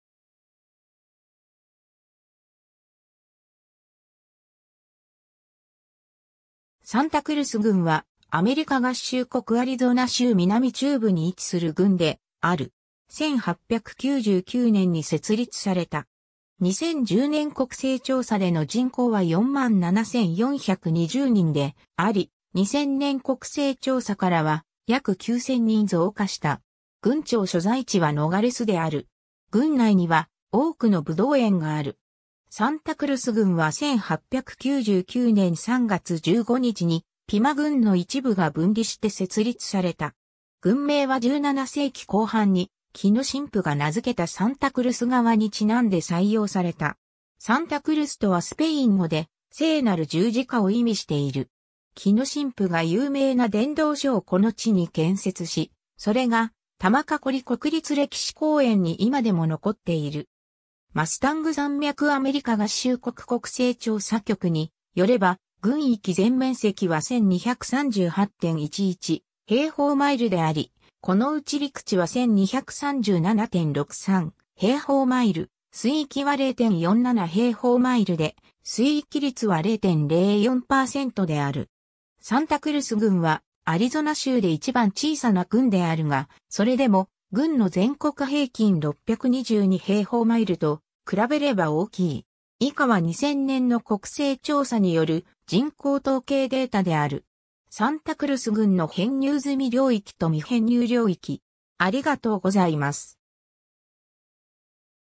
サンタクルス郡 (アリゾナ州)
サンタクルス郡（英: Santa Cruz County）は、アメリカ合衆国アリゾナ州南中部に位置する郡である。
1899年に設立された。
2010年国勢調査での人口は47,420人であり、2000年国勢調査からは約9,000人増加した。
郡庁所在地はノガレスである。
郡内には多くのぶどう園がある。
 サンタクルス郡は1899年3月15日に、ピマ郡の一部が分離して設立された。
郡名は17世紀後半にキノ神父が名付けたサンタクルス川に因んで採用された。
サンタクルスとはスペイン語で「聖なる十字架」を意味している。
キノ神父が有名な伝道所をこの地に建設し、それがタマカコリ国立歴史公園に今でも残っている。
 マスタング山脈 アメリカ合衆国国勢調査局に拠れば、郡域全面積は1,238.11平方マイル (3,206.7 km2)であり、このうち陸地は1,237.63平方マイル (3,205.4 km2)、水域は0.47平方マイル (1.2 km2)で水域率は0.04%である。
サンタクルス郡はアリゾナ州で一番小さな郡であるが、それでも郡の全国平均622平方マイル (1,610 km2)と比べれば大きい。
 サンタクルス郡の編入済み領域（灰色）と未編入領域